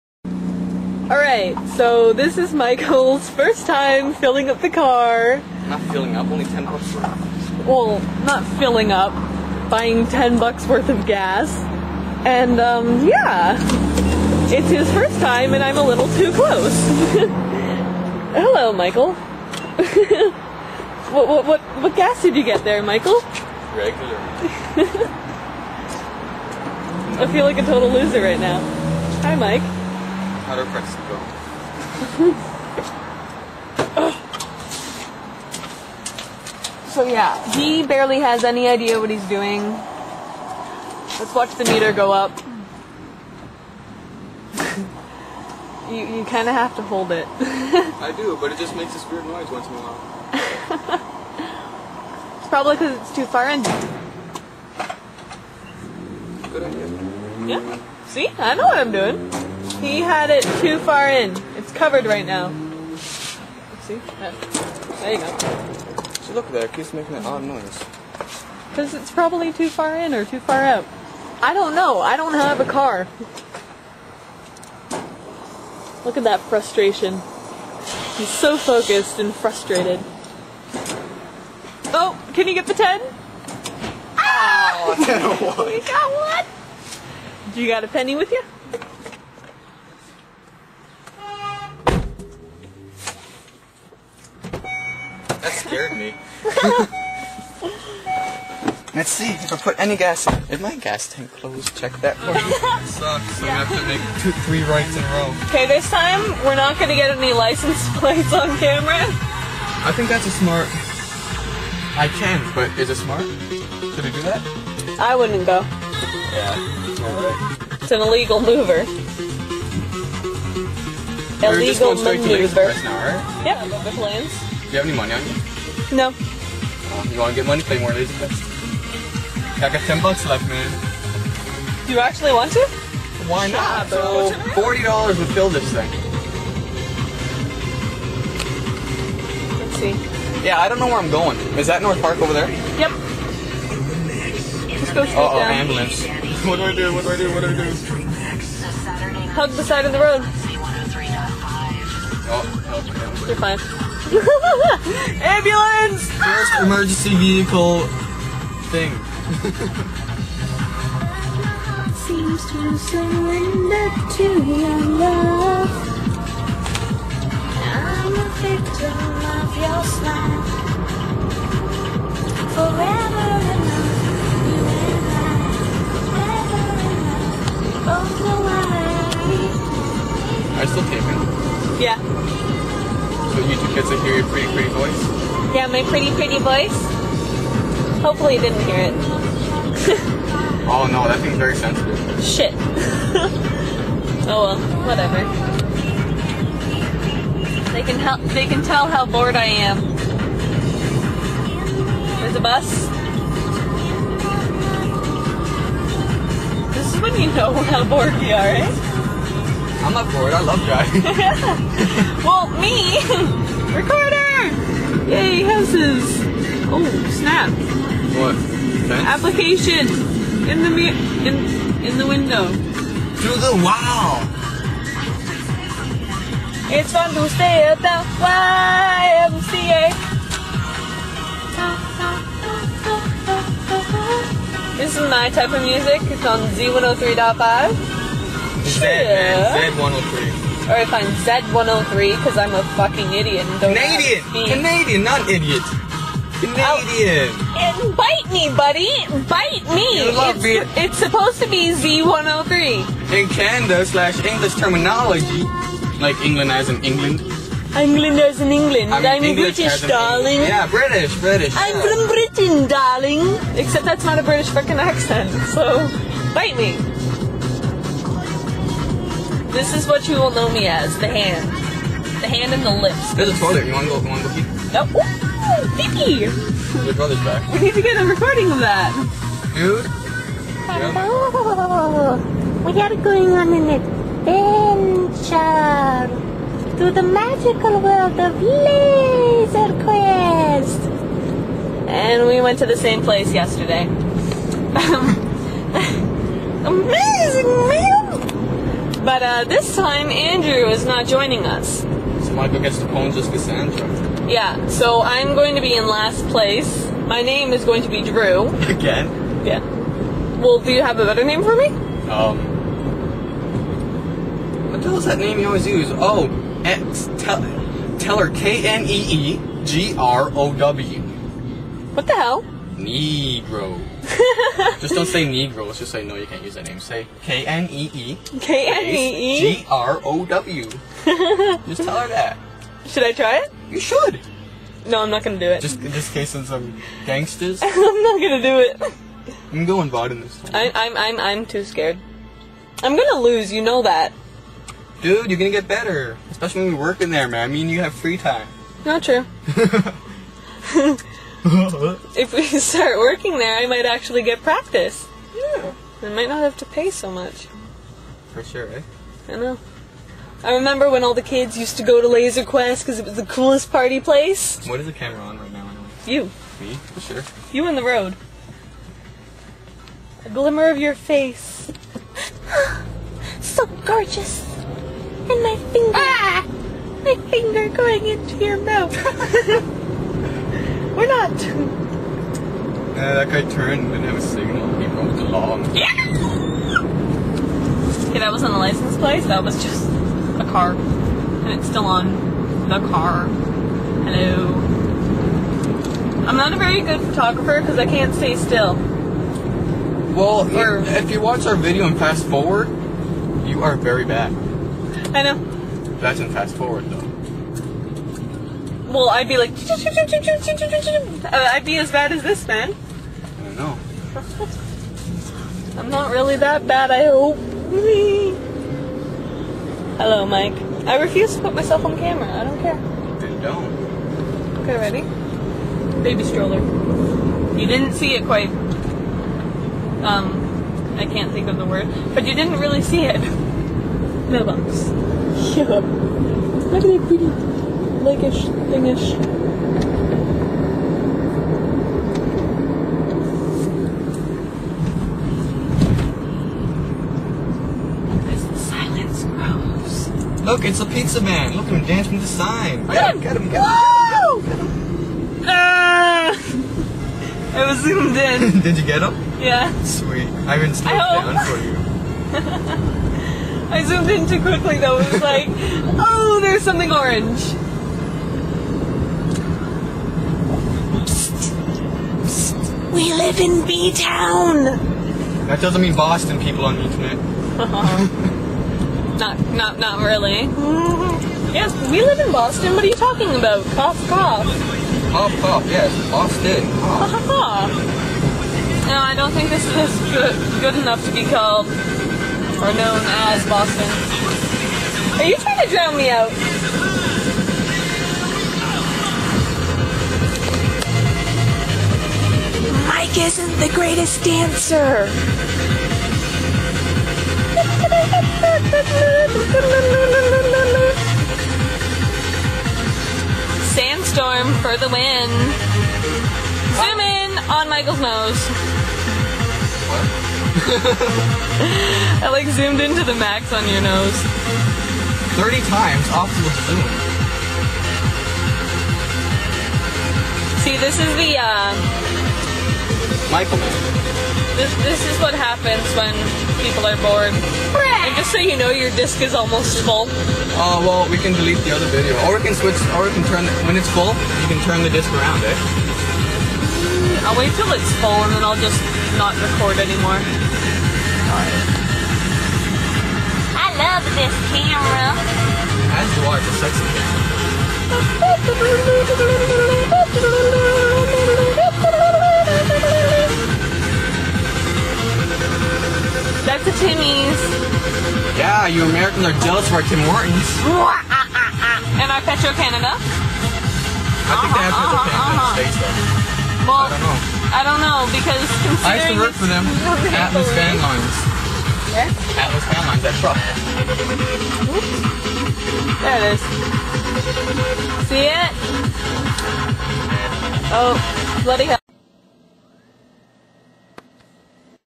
Saturday Part 1 Of 2
Cassandra and I went to hand in our applications for laser quest.